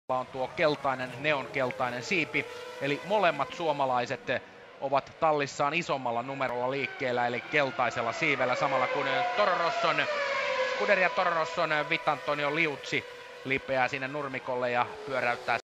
Vitantonio Liuzzi - Albert Park 2007
Vitantonio Liuzzi spins during the FP2 at Albert Park in 2007
Finnish commentary by Timo Pulkkinen & Erkki Mustakari